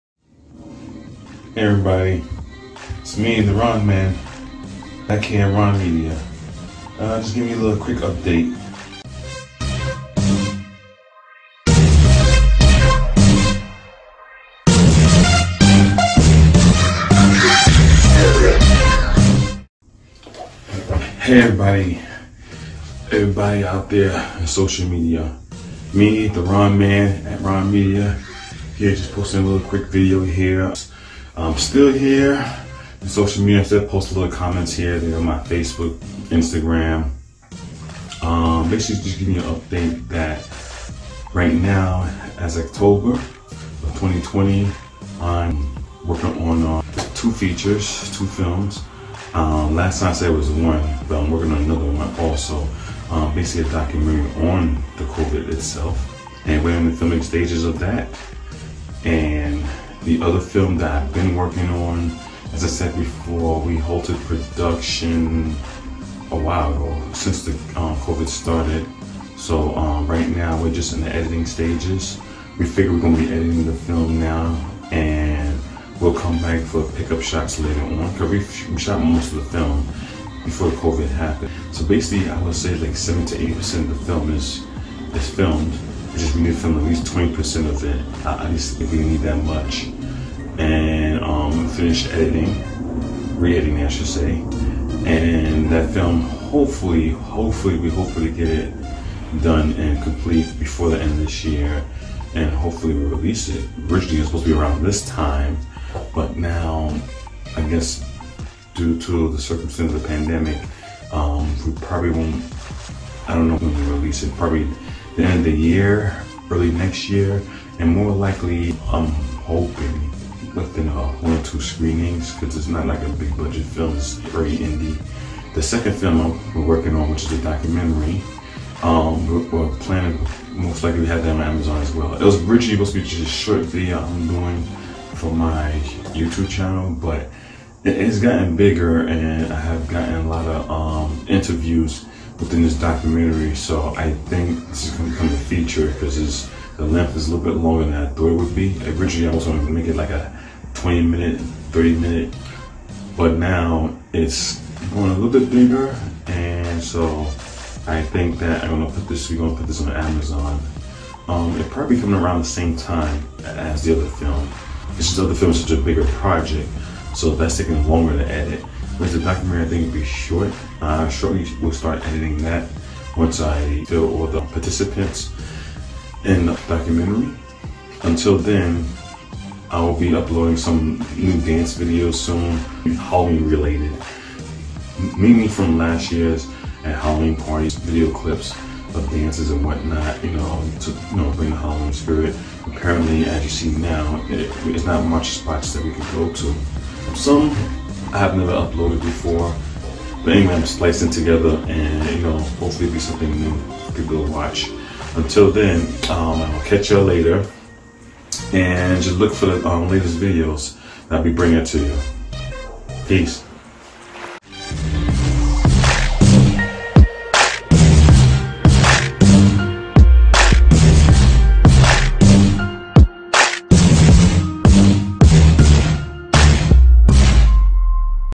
OCTOBER VIDEO BLOG UPDATE
My Video Blog for October 2020! A quick Update on What is going on in RON MEDIA!! I Discuss the reason for not posting in the last several weeks due to Film and Editing projects and whats coming to the channel for the rest of the month.